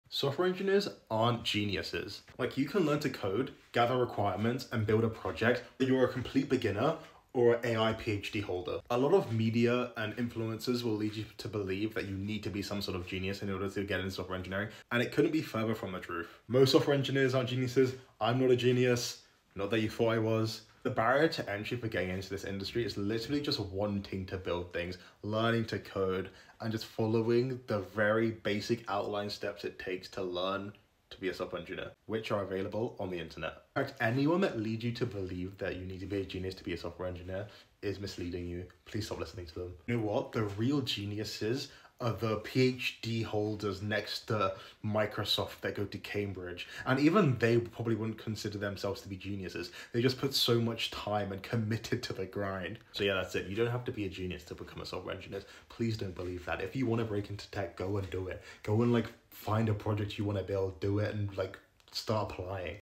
You Don’t Need to Be a Genius to Code 💻
Every pro coder started as a beginner who refused to quit. If you love solving problems and building things, you already have what it takes. 💪